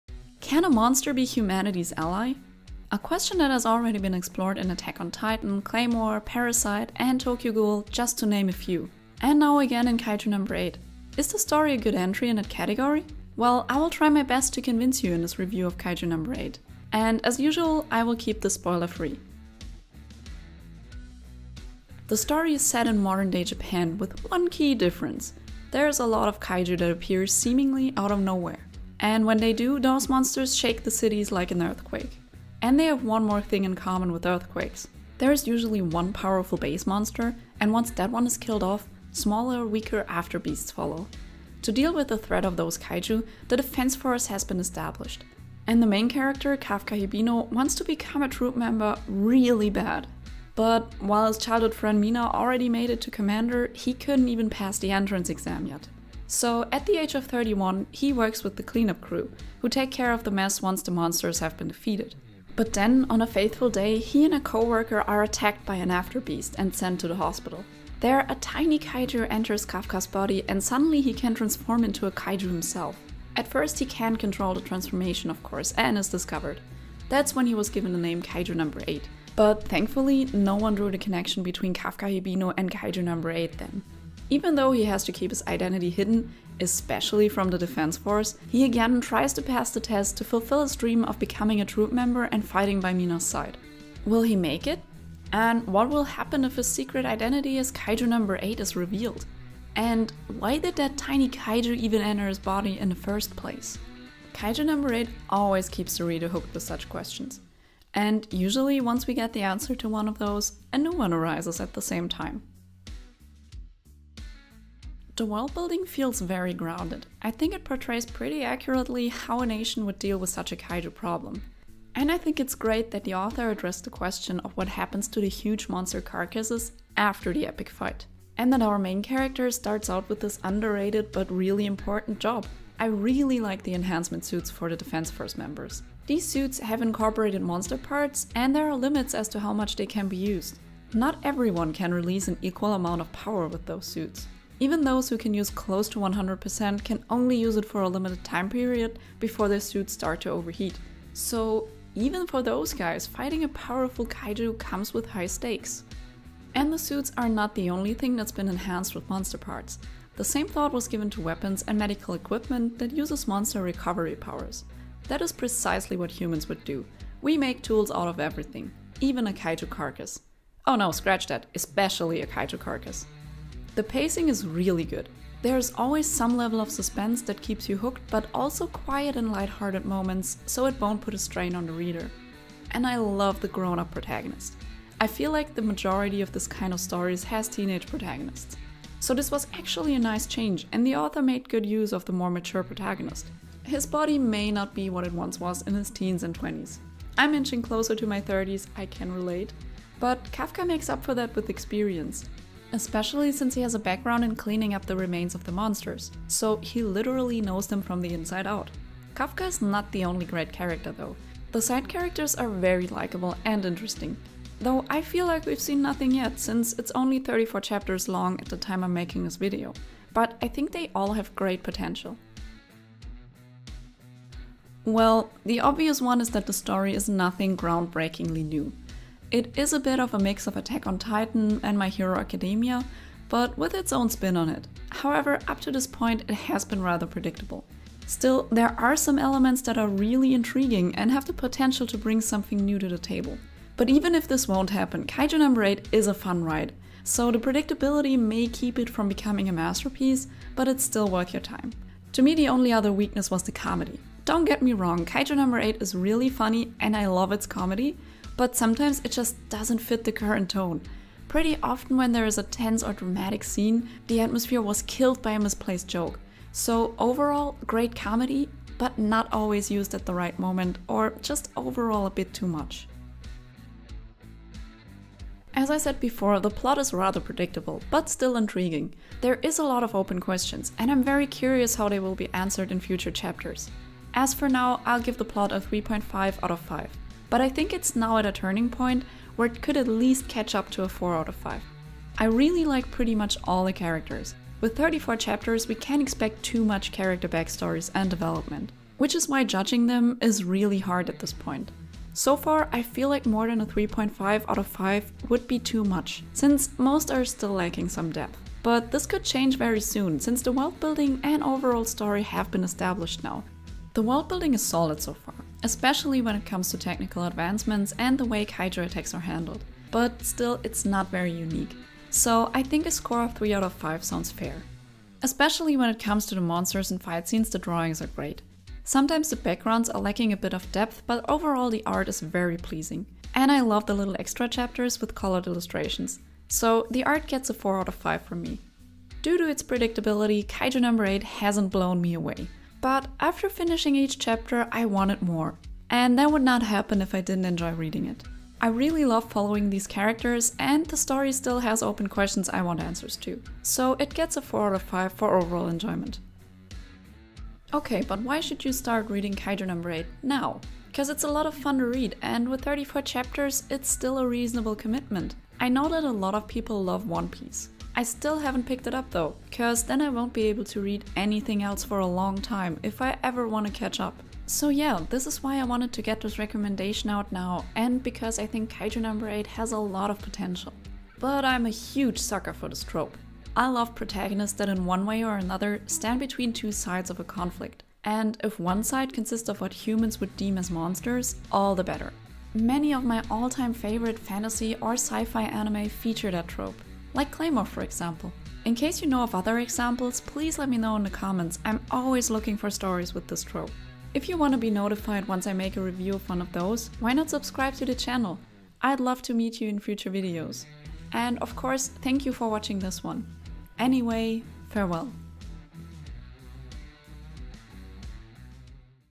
Why you should read Kaiju No. 8 NOW
This is a spoiler-free review of the manga Kaiju No. 8 by Naoya Matsumoto. In this manga Kaiju plague Japan with frequent attacks. Soon the main character Kafka Hibino  finds himself in the position that he can change into a Kaiju himself: Kaiju No. 8. Can he still make his dream of becoming a defense force member a reality now?

0:00 Intro
0:20 Set-Up
1:53 Strengths
3:43 Weaknesses
4:33 Conclusion

For this speedpaint I used panels from the manga as reference.